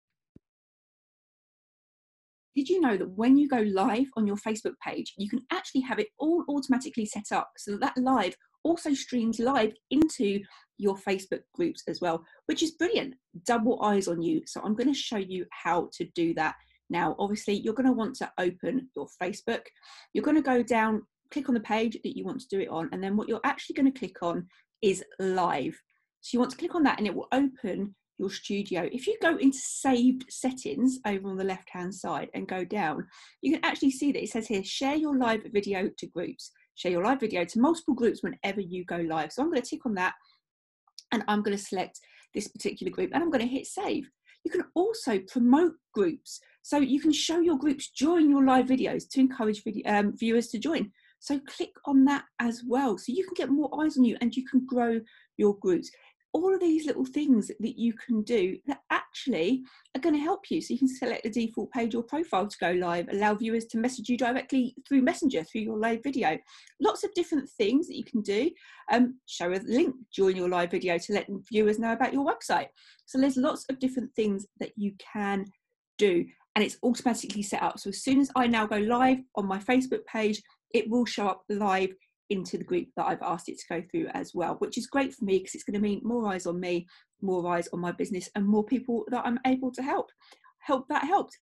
How to stream your facebook live into your groups automatically
Share your live video to multiple groups whenever you go live.

If you love the free content I provide on my YouTube channel and find it helpful feel free to make a donation of any amount you would like, to say thanks